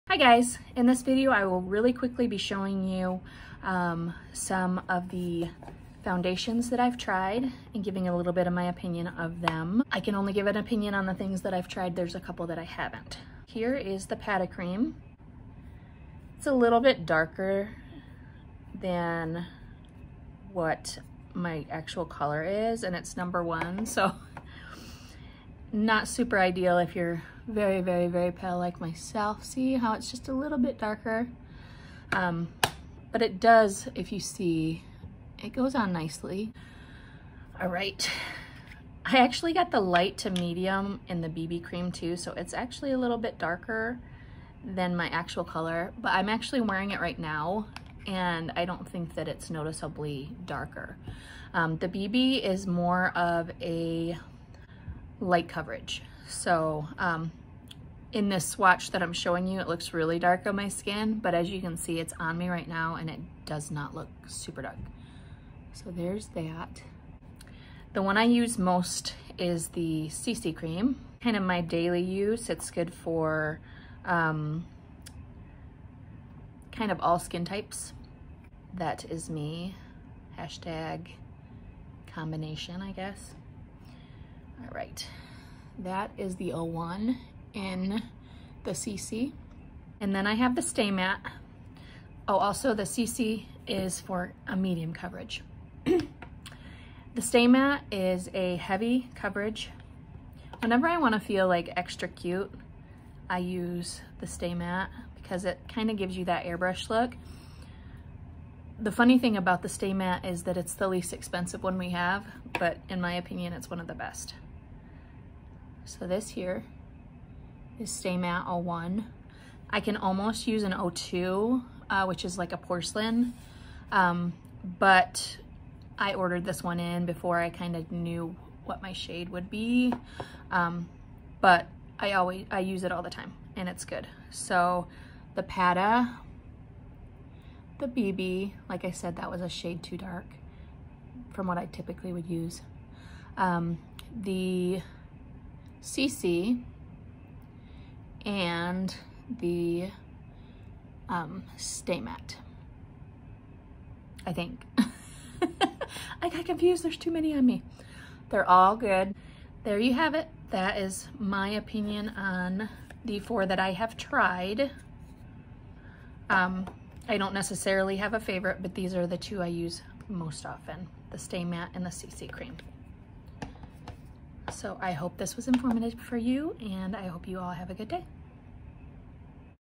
My Opinion: Foundation Edition
My honest opinions of Farmasi foundations.